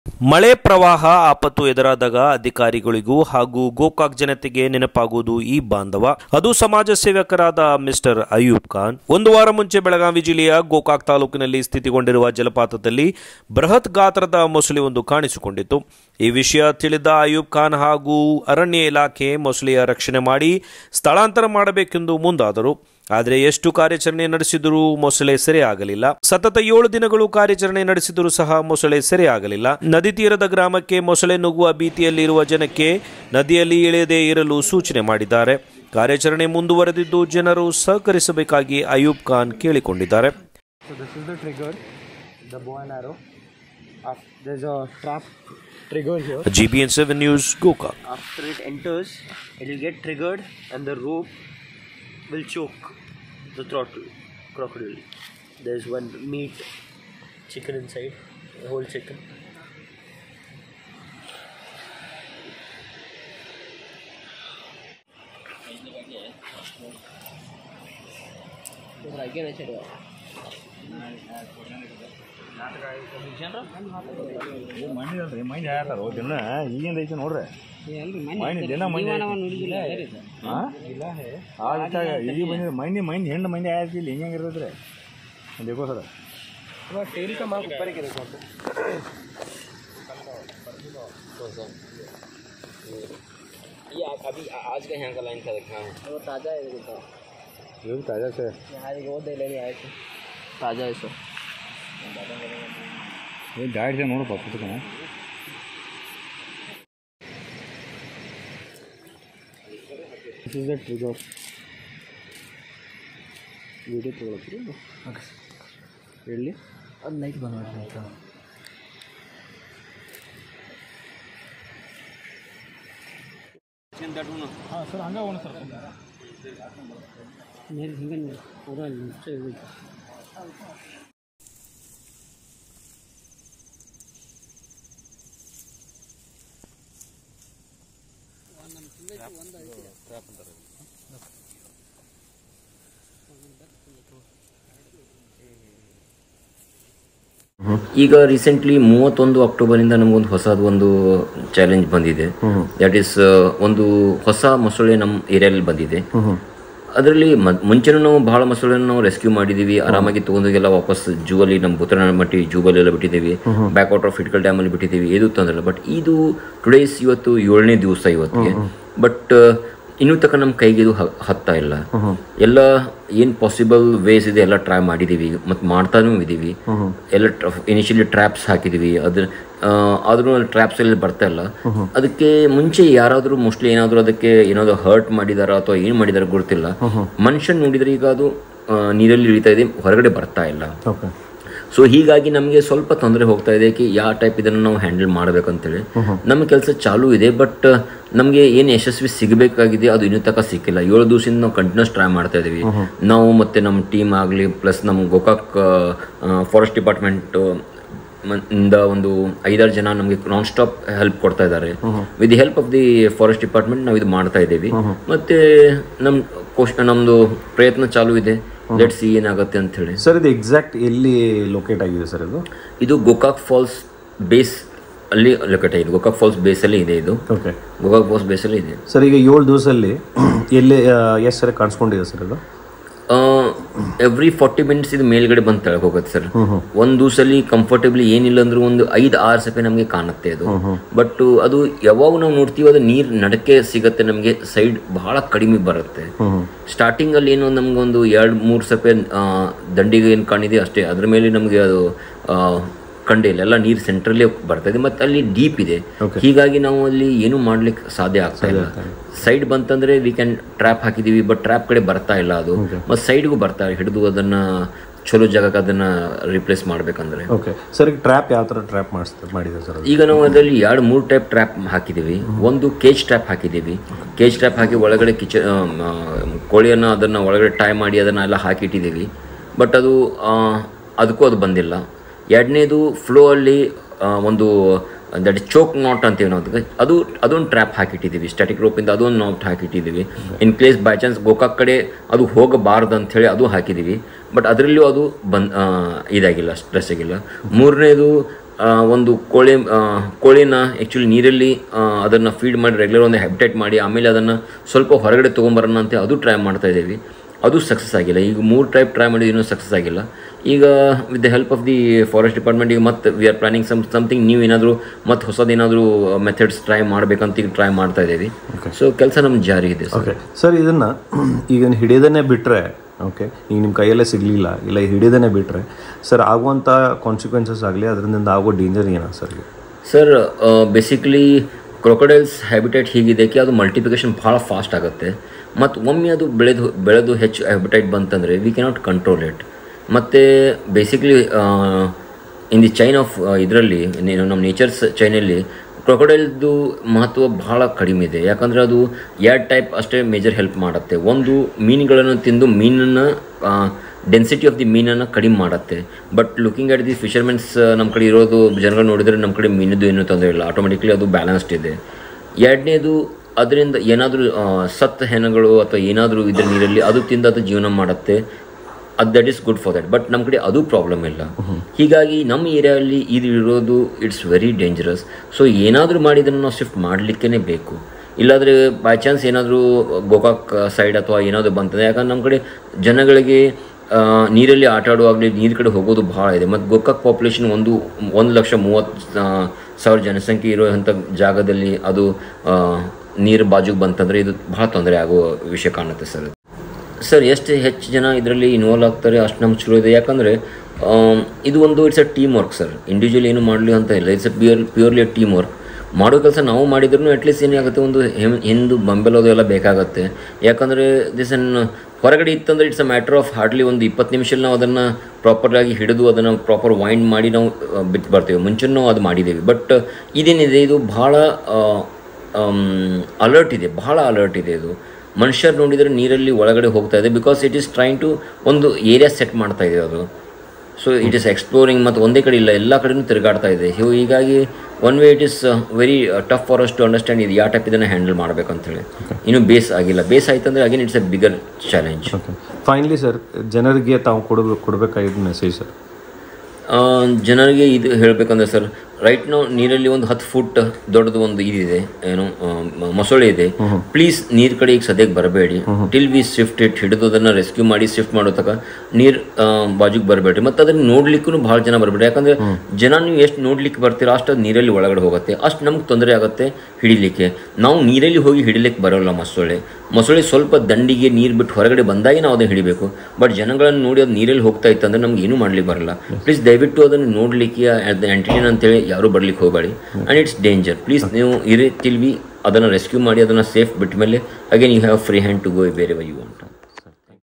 7 ದಿನ ಕಾರ್ಯಾಚರಣೆ ನಡೆಸಿದರೂ, ಸೆರೆಯಾಗದ ಮೊಸಳೆ : ನದಿ ತೀರದ ಗ್ರಾಮಕ್ಕೆ ಮೊಸಳೆ ನುಗ್ಗುವ ಭೀತಿ..!!!
7 ದಿನ ಕಾರ್ಯಾಚರಣೆ ನಡೆಸಿದರೂ, ಸೆರೆಯಾಗದ ಮೊಸಳೆ : ನದಿ ತೀರದ ಗ್ರಾಮಕ್ಕೆ ಮೊಸಳೆ ನುಗ್ಗುವ ಭೀತಿ